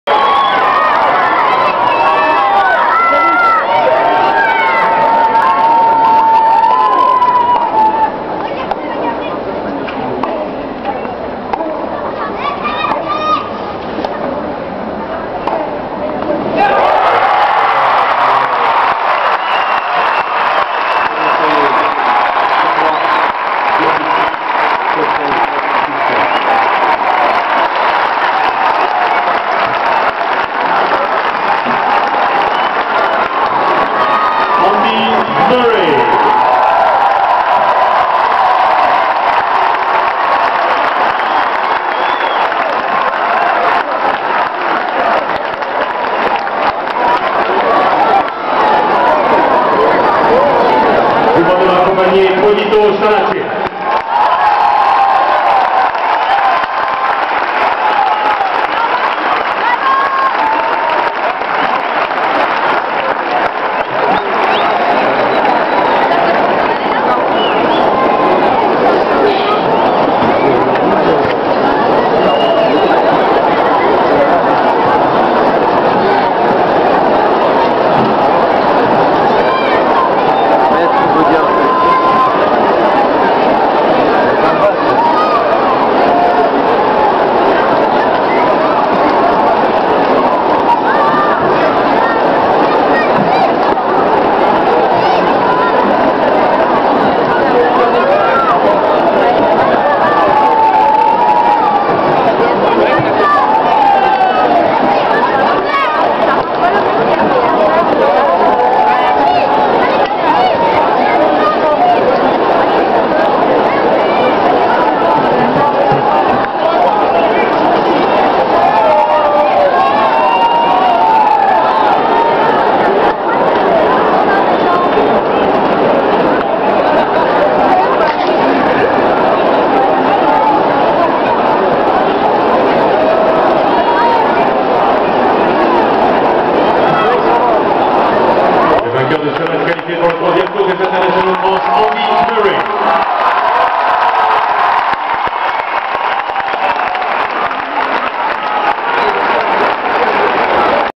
Rolland Garros : balle de match entre Andy murray et p.starace
Court Phillippe Chatrier, le 27 mai 2009

La balle de match entre Andy Murray et P.Starace